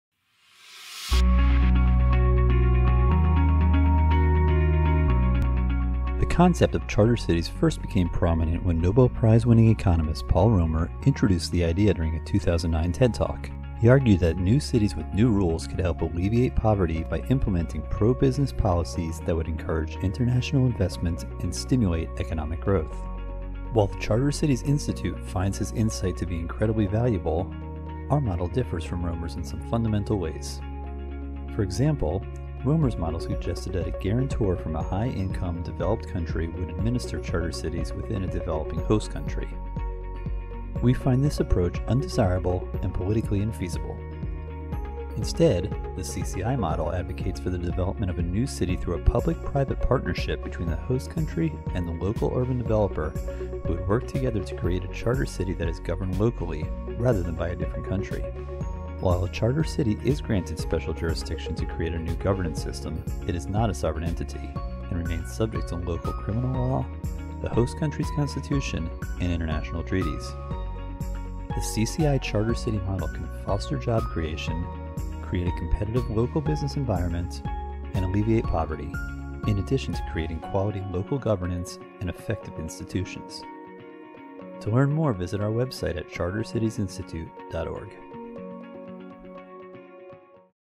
What Makes CCI’s Charter City Model Different
How our vision of charter cities differs from other models

The concept of charter cities was introduced by Nobel Prize-winning economist Paul Romer during his TED talk in 2009. The Charter Cities Institute was inspired by Romer, but our charter cities model is different.